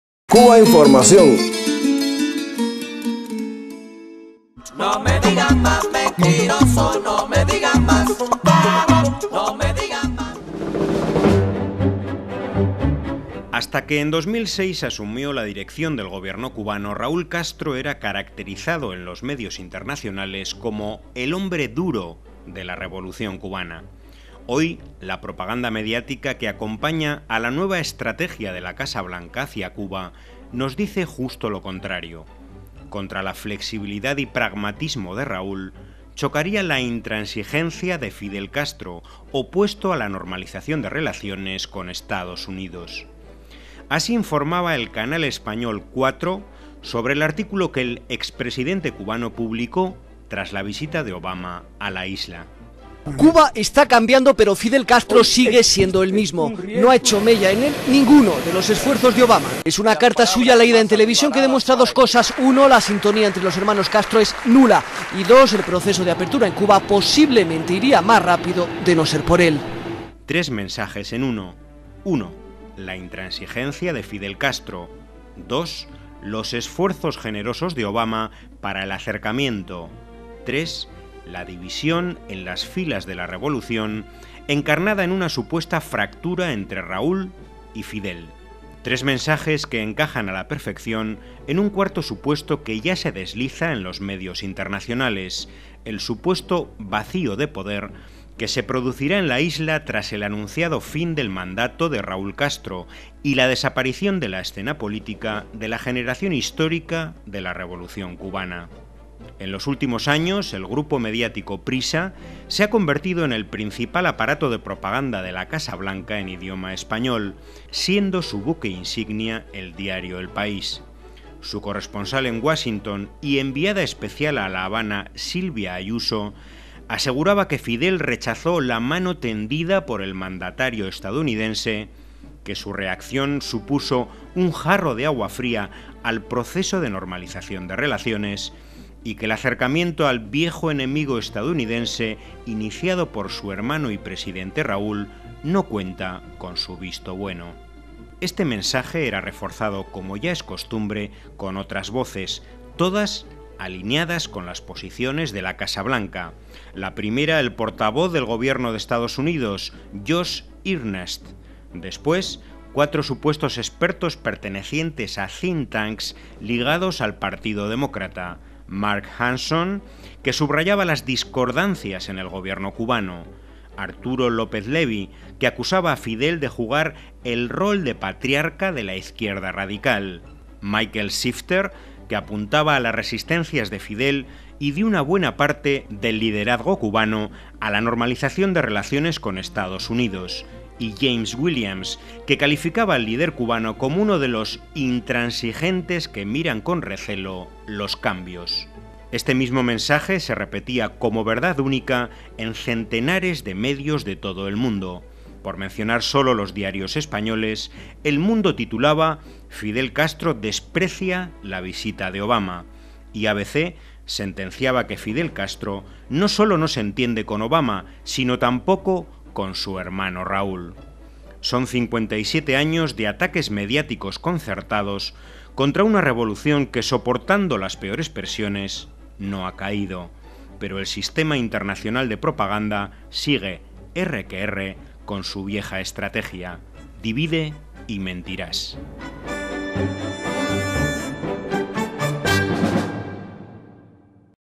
Fidel y Raúl Castro: divide y mentirás... pero no vencerás
Los principales medios nos repiten tres mensajes en uno: uno, la intransigencia de Fidel Castro; dos, los esfuerzos generosos de Obama para el acercamiento; tres, la división en las filas de la Revolución, encarnada en una supuesta fractura entre Raúl y Fidel. Tres mensajes que encajan a la perfección en un cuarto supuesto que ya se desliza en los medios internacionales: el “vacío de poder” que se producirá en la Isla, tras el anunciado fin del mandato de Raúl Castro y la desaparición de la escena política de la generación histórica de la Revolución cubana. Edición: Esther Jávega.